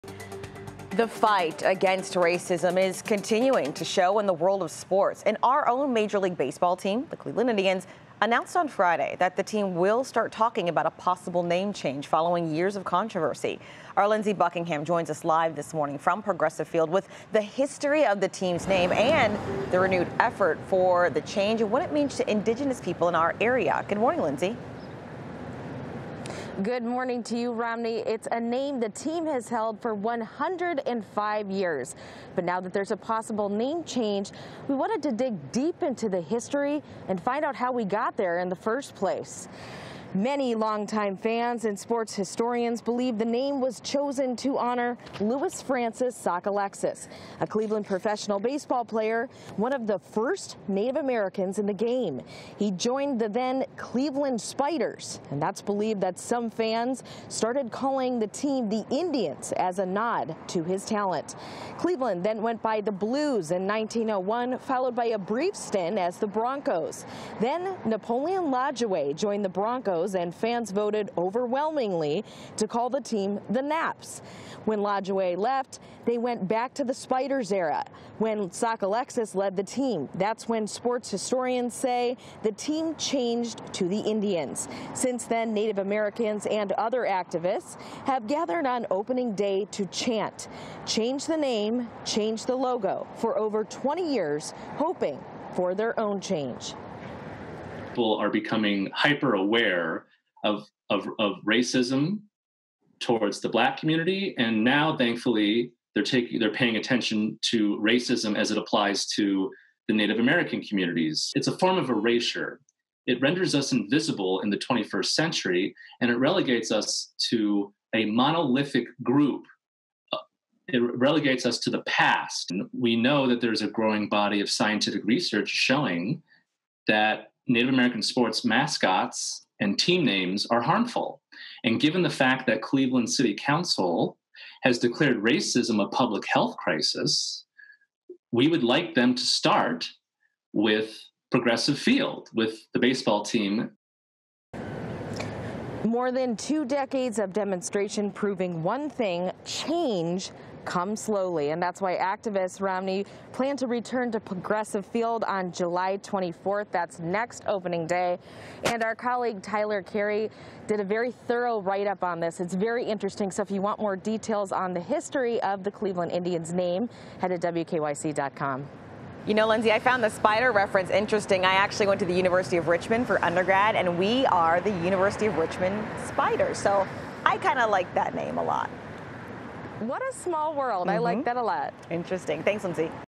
The history of the name 'Cleveland Indians'
The team may be changing its identity. Lindsay Buckingham reports.